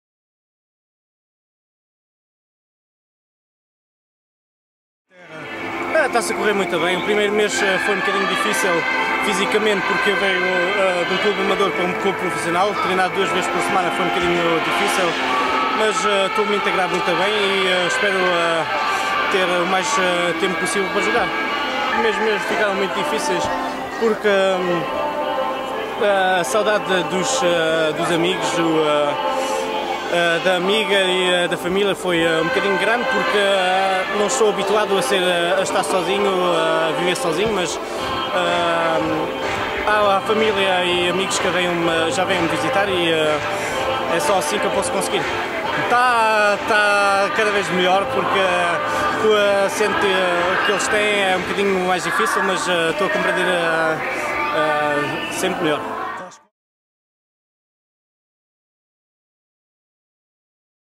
Yannick Bastos sobre a vida no Bolton
O luxotuga jogador do Bolton explica ao BOMDIA como foram as primeiras semanas em Inglaterra e fala da integração no clube e no país.